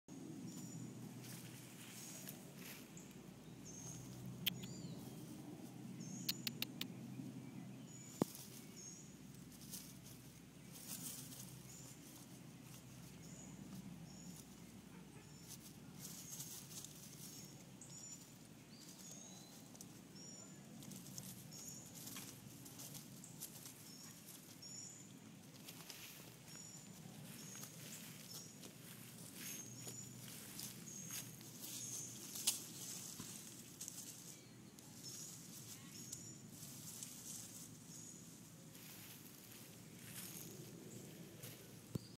Armadillo
I've never seen a live, wild armadillo. He was eating ants, I think.